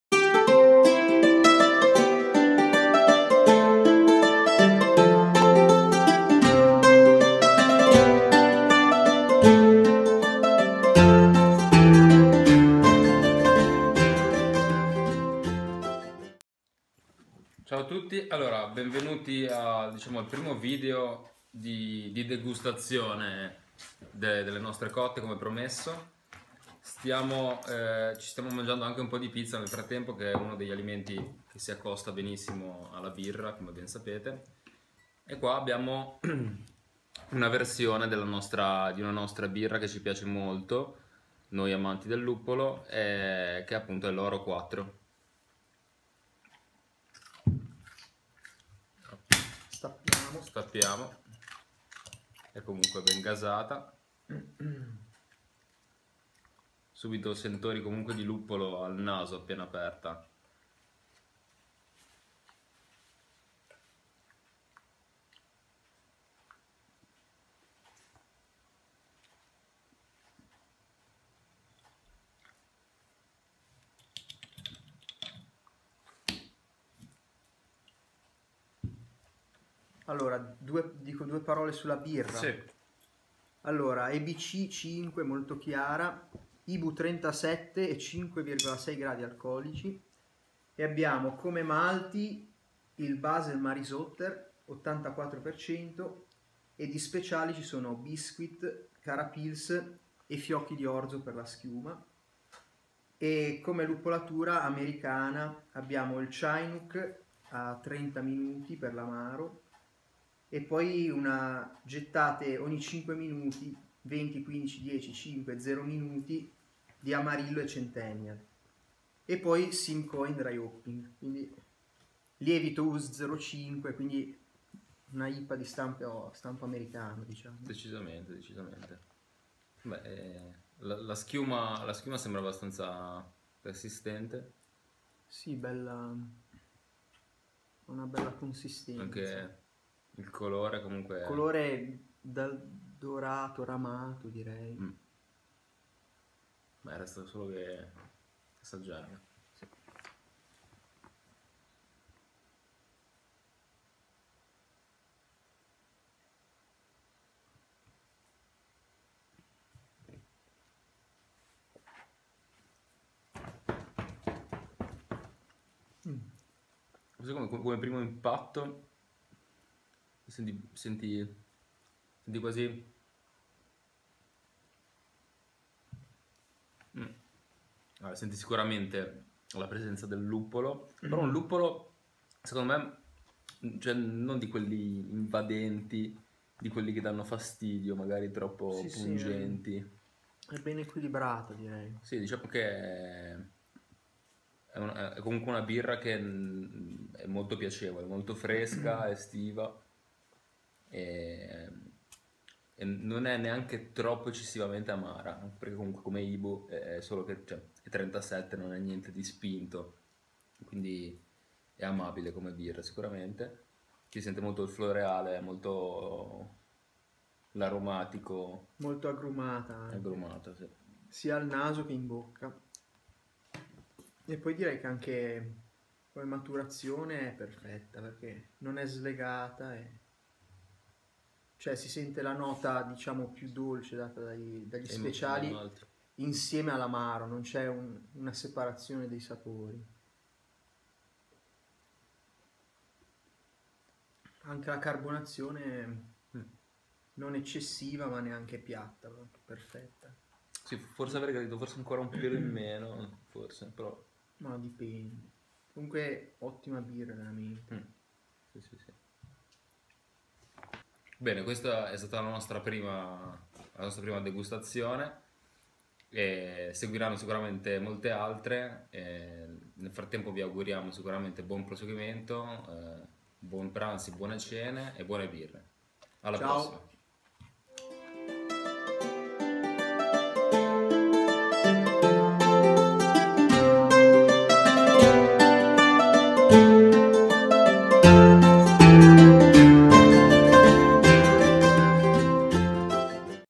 Signormalto Degustazione American Pale Ale Oro 4
Primo video di degustazione. APA 4ª versione di questa splendida bionda luppolata.
http:signormalto.blogspot.it